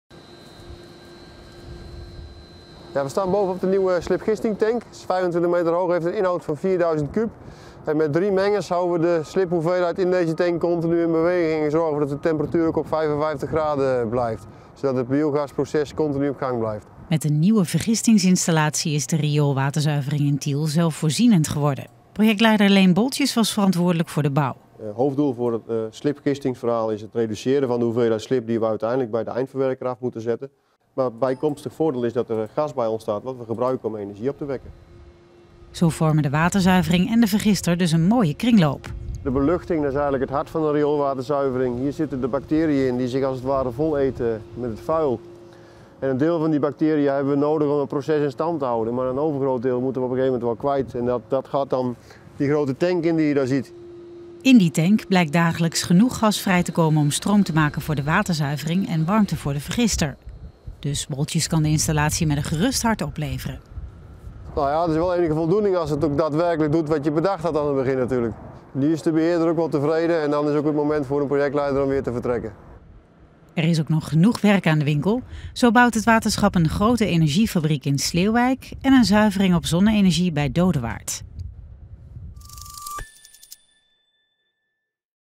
Nieuwe vergister maakt Tielse waterzuivering tot energiefabriek
TIEL - Waterschap Rivierenland wil energie-neutraal worden. Daarom is er bij de rioolwaterzuivering in Tiel een nieuwe vergistingsinstallatie gebouwd. Deze blijkt voldoende energie op te leveren om zelfvoorzienend te zijn. Het complex is daarom nu officieel omgedoopt tot energiefabriek.